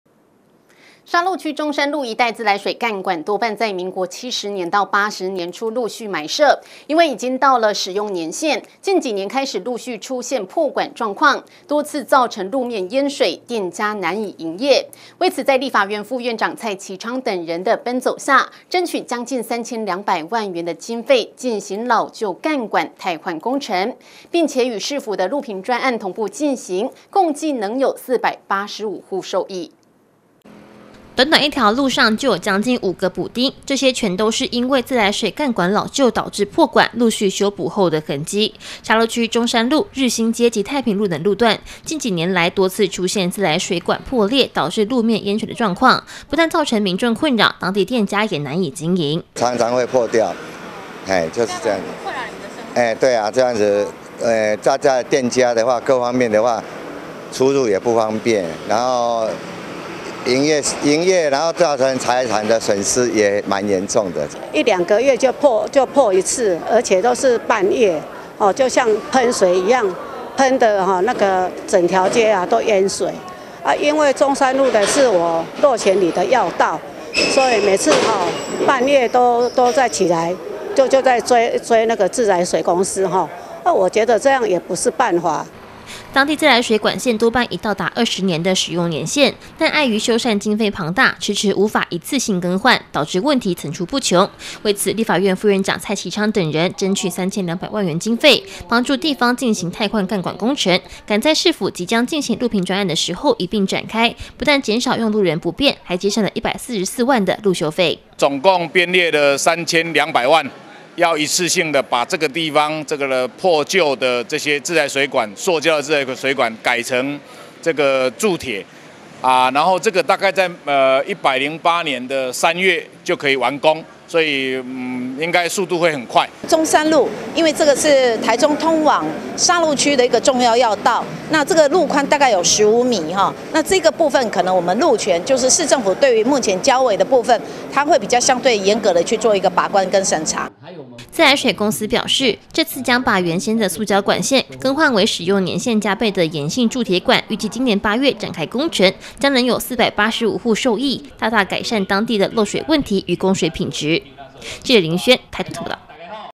大台中新聞-中山路自來水常爆管 8月汰換幹管改善
沙鹿區中山路一帶自來水幹管，多半在民國70至80年初陸續埋設，因為已到達使用年限，近幾年開始陸續出現破管狀況，多次造成路面淹水，店家難以營業，為此，在立法院副院長蔡其昌等人的奔走下，爭取近3200萬經費進行老舊幹管太換工程，並與市府路平專案同步進行，共計能有485戶受益。。。。。「以上新聞可從top NEWS APP收視」